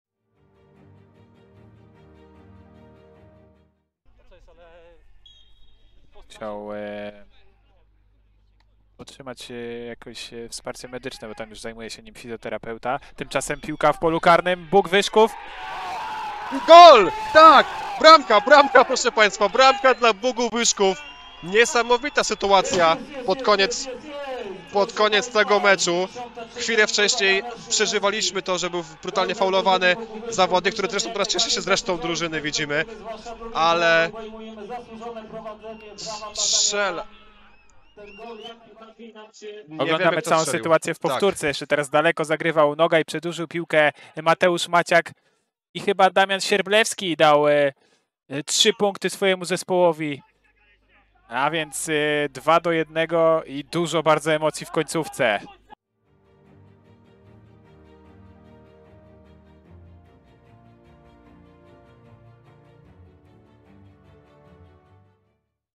GOL W DOLICZONYM CZASIE GRY!! Damian Świerblewski na 2:1 | Bug Wyszków - KS Łomianki
Mecz Ligi okręgowej, 19 kolejka: Bug Wyszków - KS Łomianki.

Trafienie Damiana Świerblewskiego w 96 minucie spotkania.